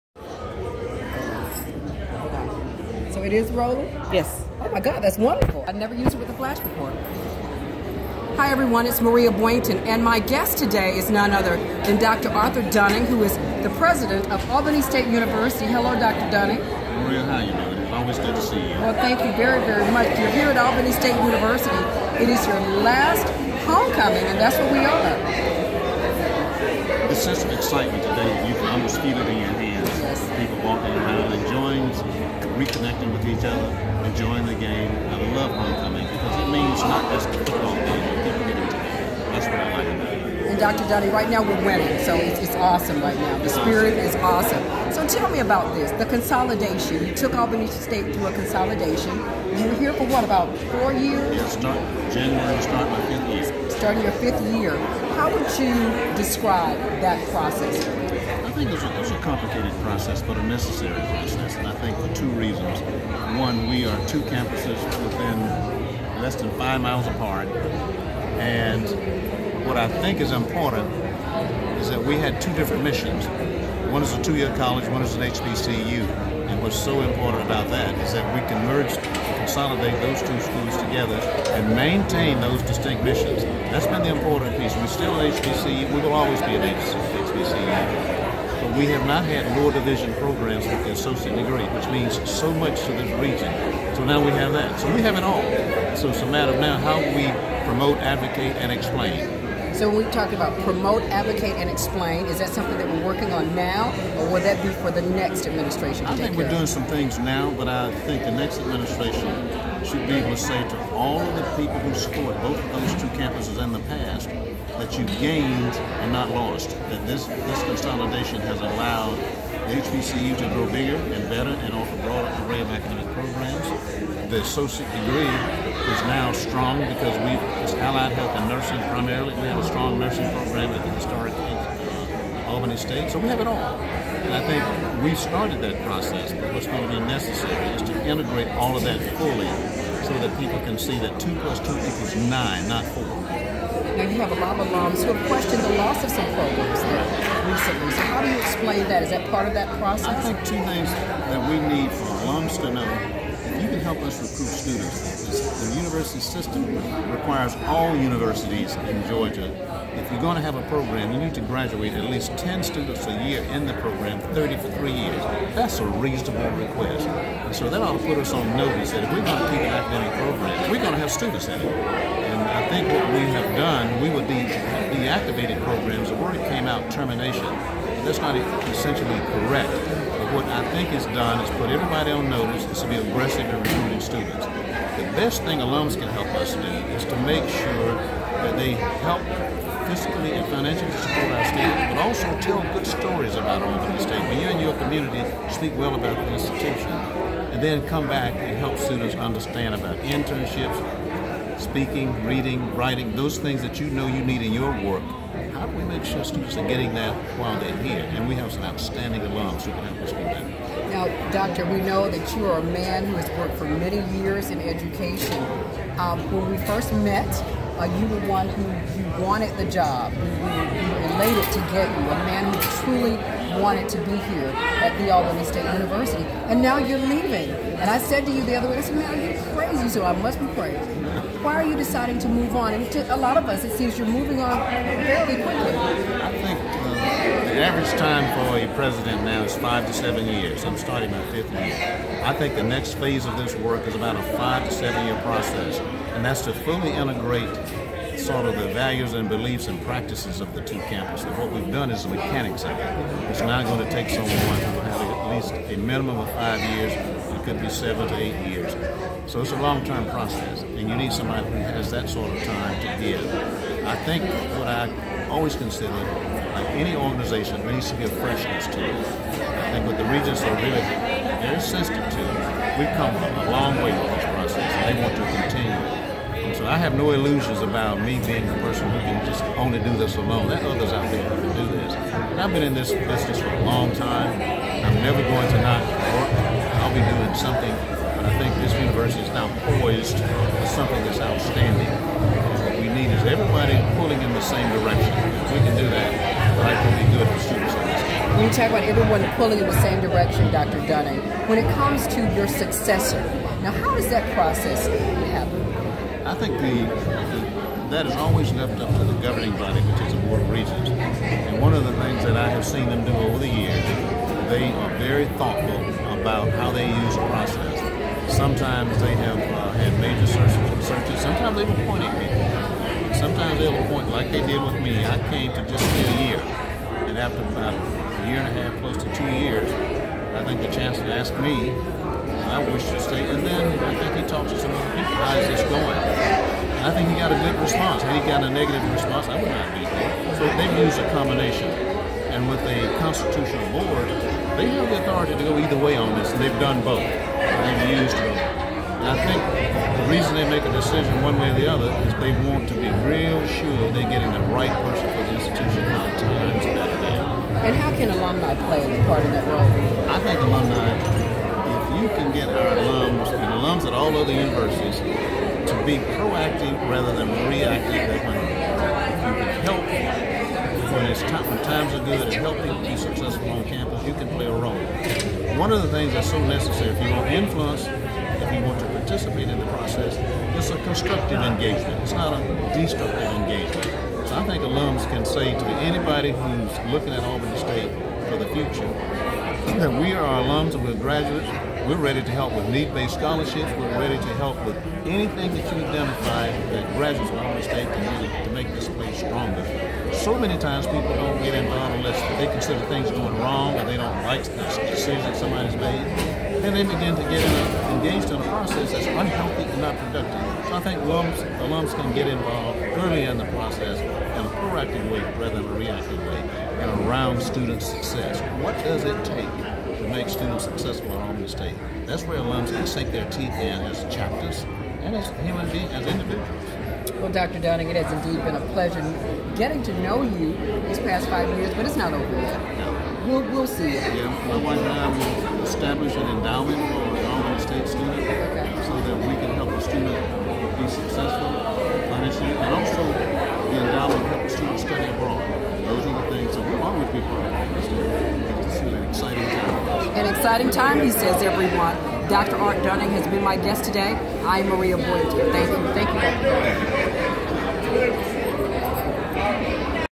Albany State University President Dr. Arthur Dunning
Dunning talks about the Consolidation and his Retirement